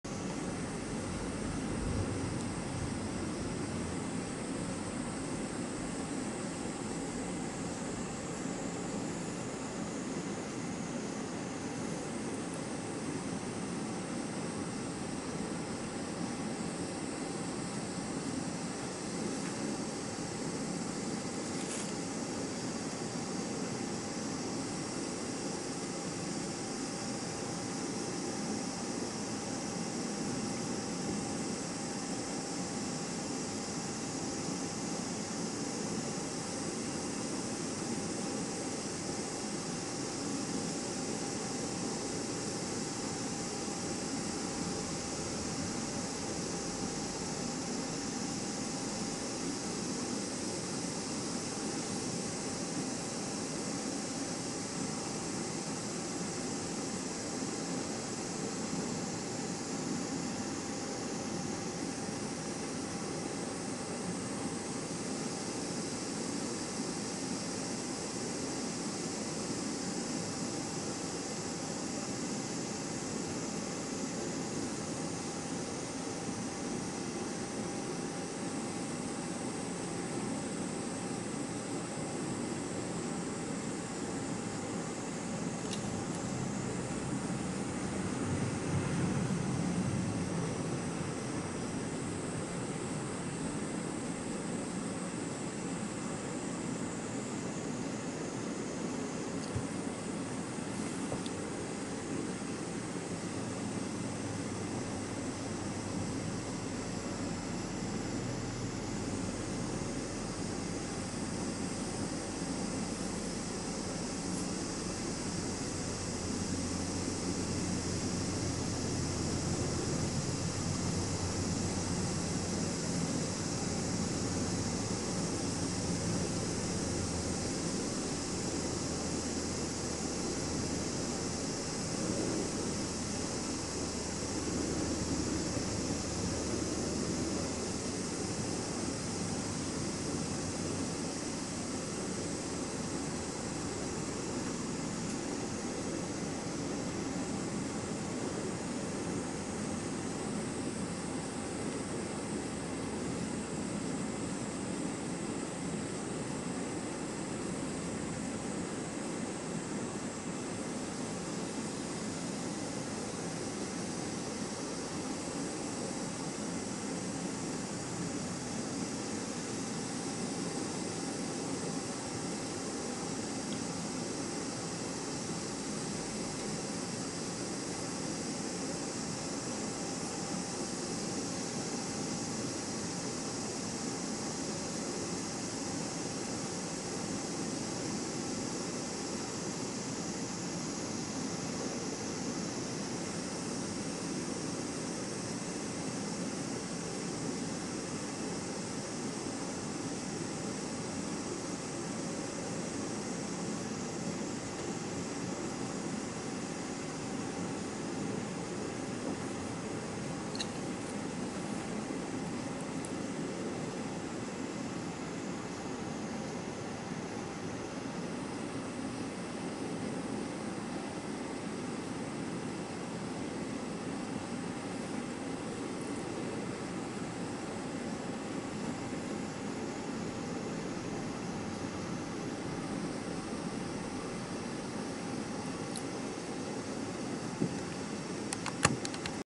ハナダカバチの繭作り（その２）
ハナダカバチの幼虫は卵から孵化して約７日目に繭を作り繭の中に入ります。もう餌は食べません。繭の中に入った幼虫は取り込んだ砂で体が傷つかないように糸で幕を作りセイム皮のようにします。これは繭の中に幕を作っているところです。